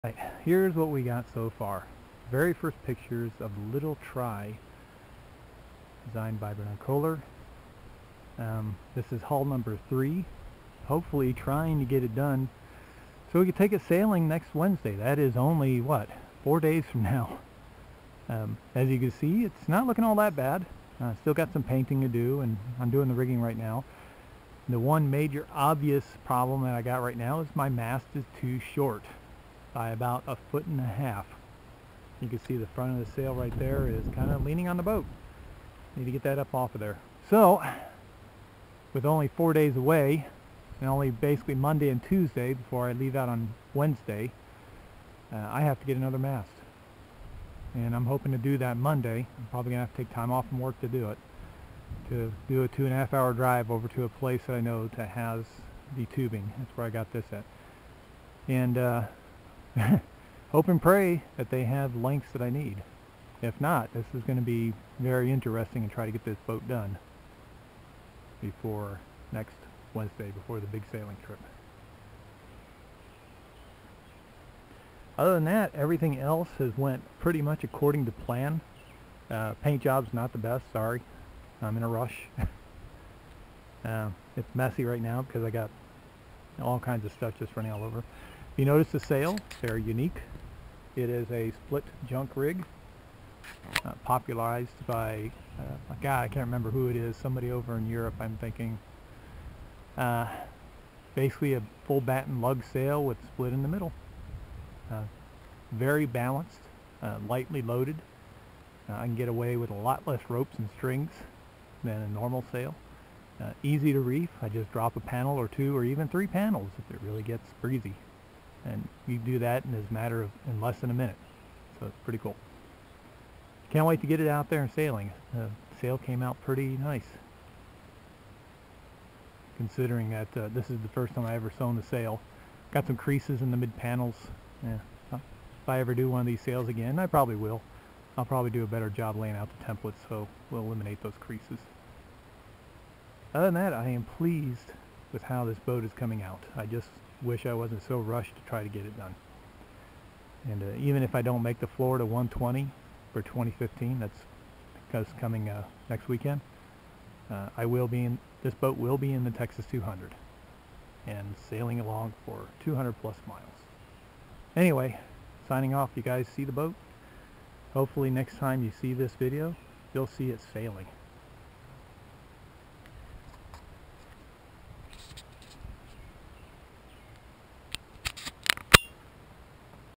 Little Tri by Mike
Designed by Bernd Kohler, the Little Tri is a 15.4' long outrigger trimaran design made from plywood and fir wood.  This is hull #3 worldwide and will hopefully splash in less than 5 days from now.

At this point, the boat is sitting in my driveway getting the sail hung for the first time and rigging added.  Still some painting yet to do too.  :)